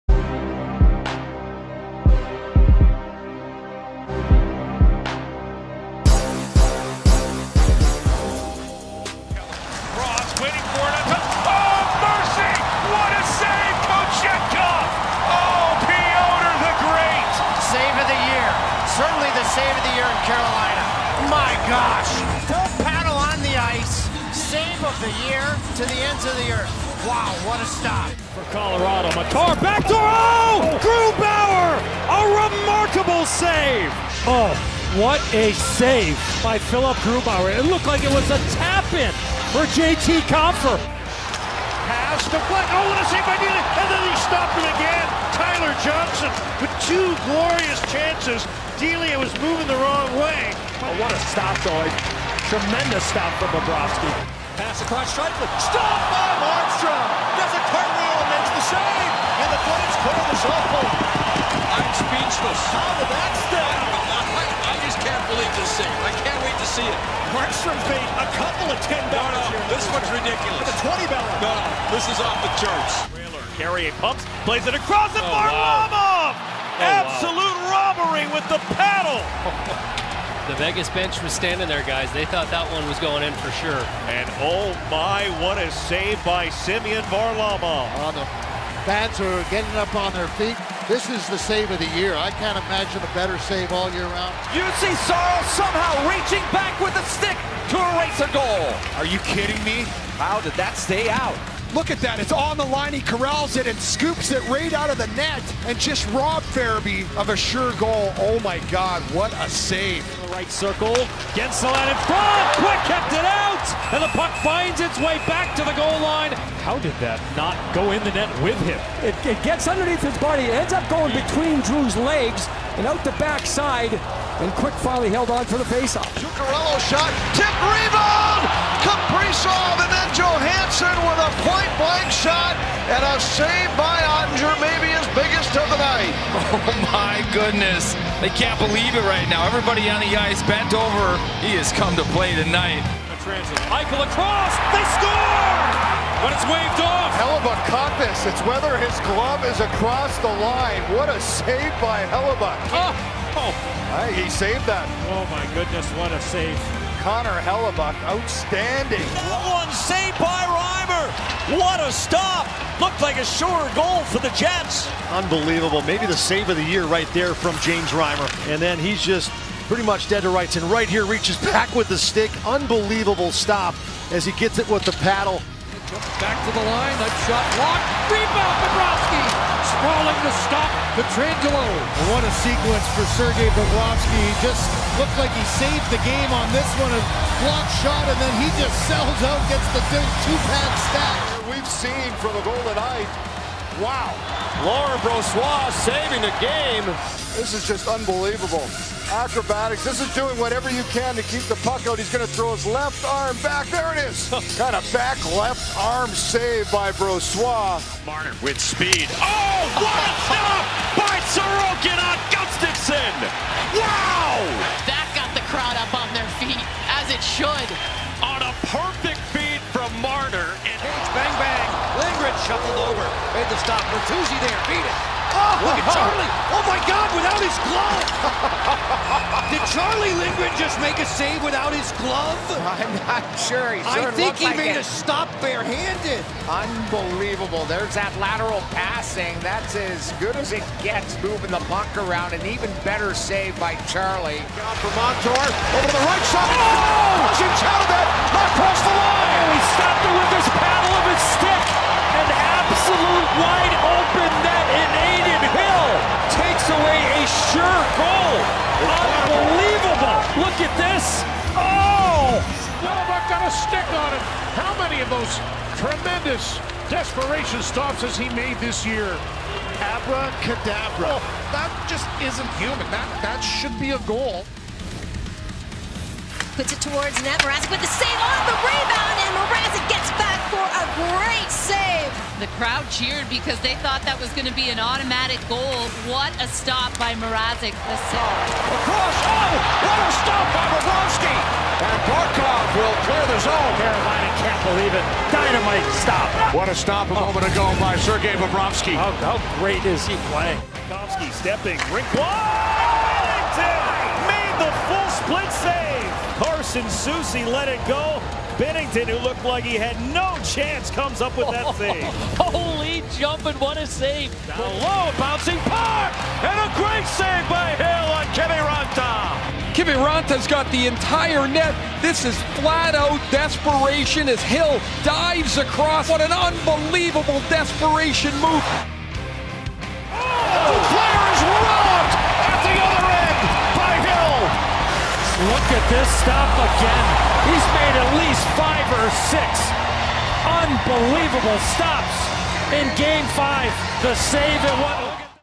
The Best Saves of the 2022-2023 NHL Season
From incredible desperation paddle stops to jaw-dropping toe saves, the NHL's goaltenders put on a show during the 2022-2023 season! Here are all of the best saves from the campaign in one video!